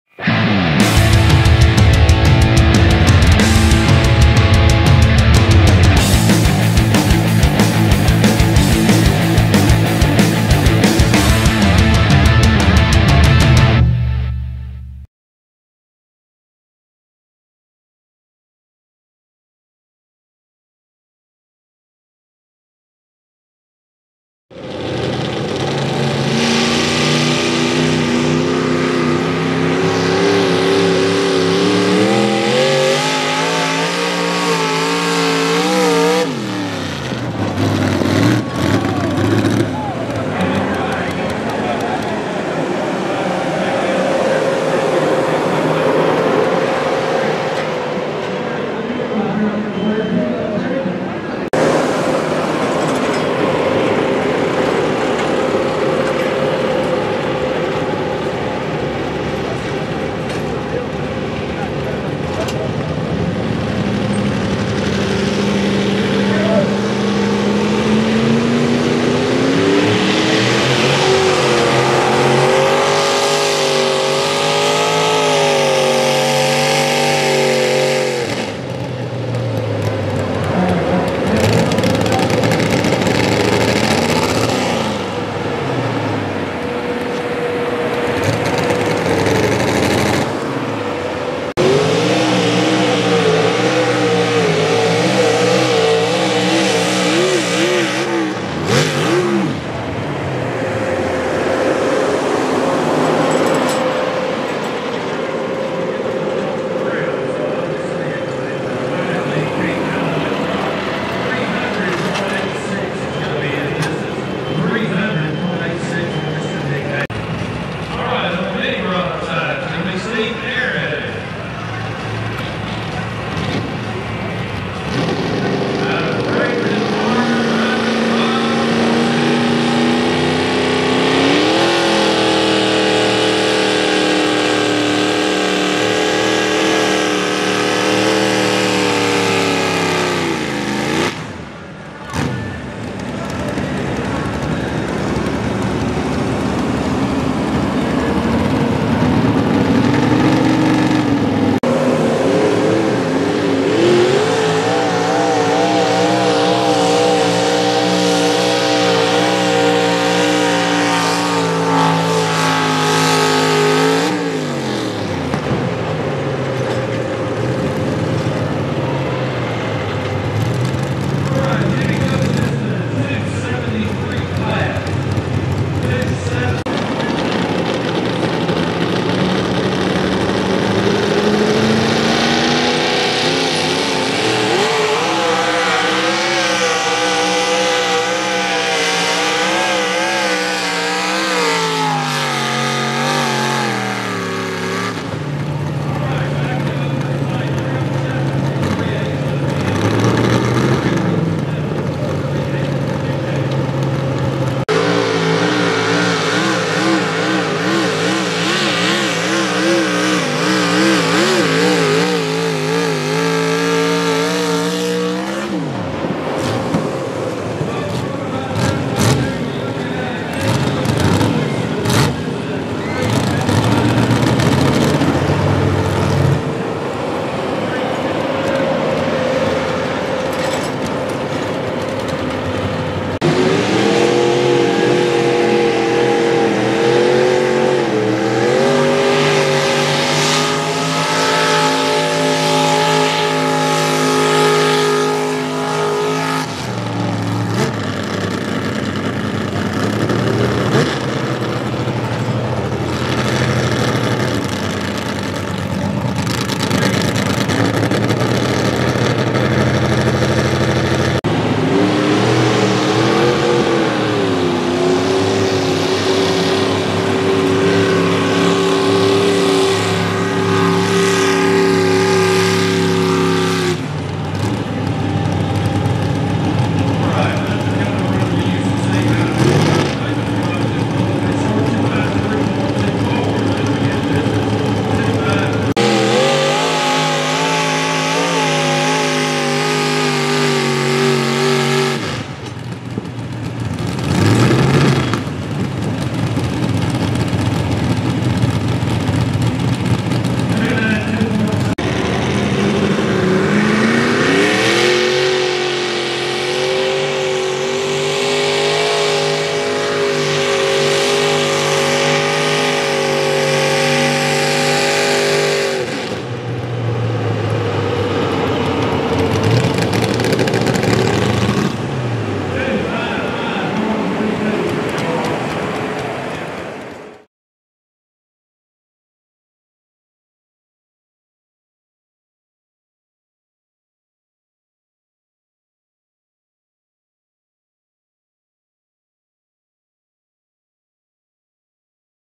2023 Jackson, TN Mini Rod Tractor Pulling
Mid-South Pullers Association
Mini Rod Tractors
Jackson Fairgrounds Park - Jackson, TN
Friday June 9, 2023